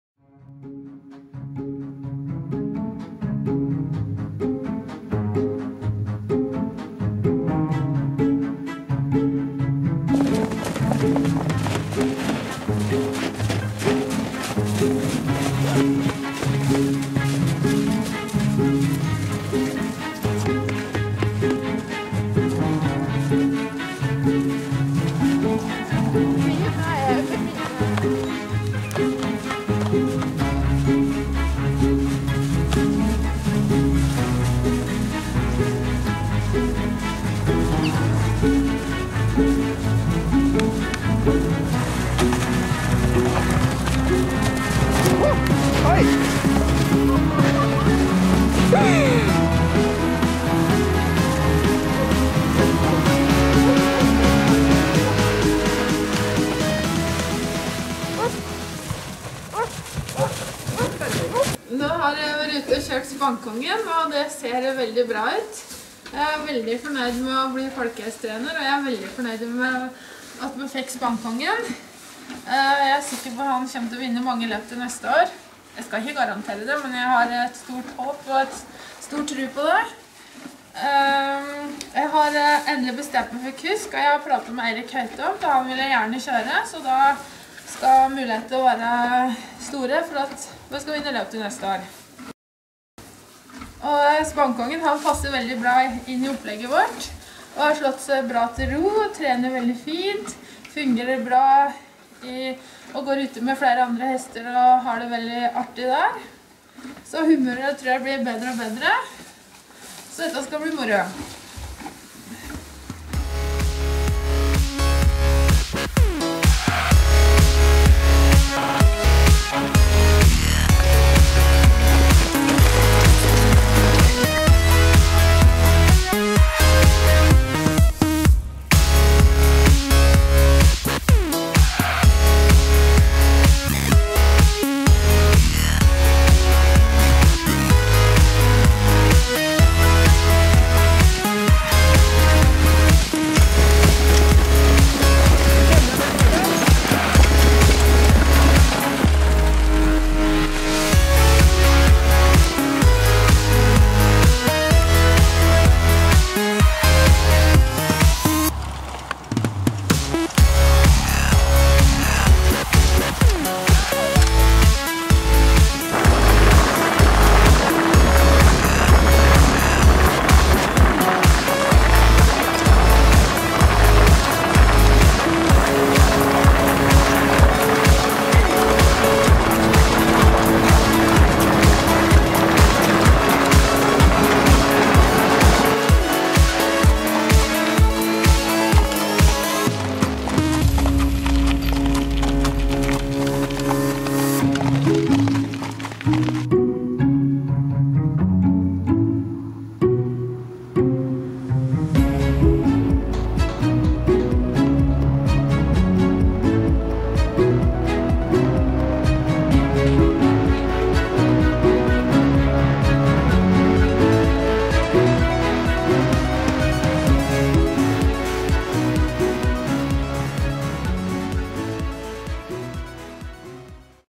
Folkehesten Biri 2016 - Spang Kongen 10.desember 2015
Spang Kongen ble godkjent folkehest for Biri etter undersøkelse på Biri hesteklinikk. Så bar det til Begnadalen og trener Heidi Moen. Dette er en liten hilsen fra henne og Spang Kongen :D